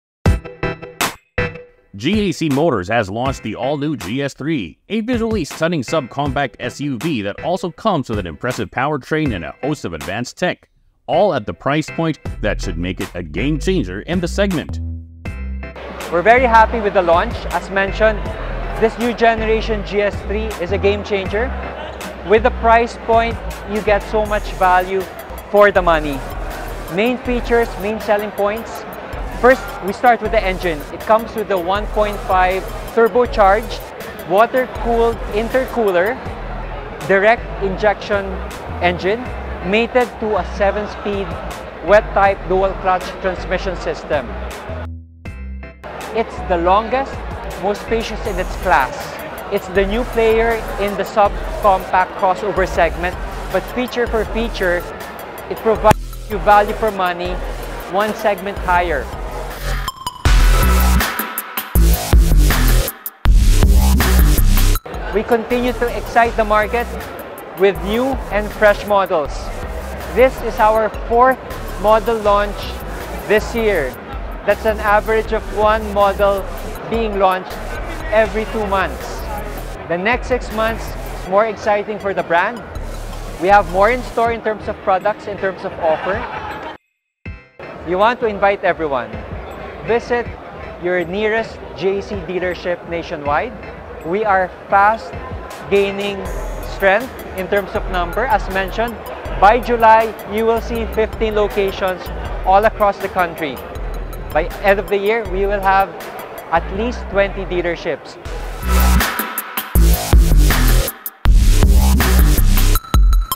GAC Launches Game-changing All New GS3 | Auto Industry News
GAC Motors has launched the All New GS3 Emzoom, a visually stunning subcompact SUV that also comes with an impressive powertrain and a host of advanced tech, all at a price point that should make it a game changer in the segment.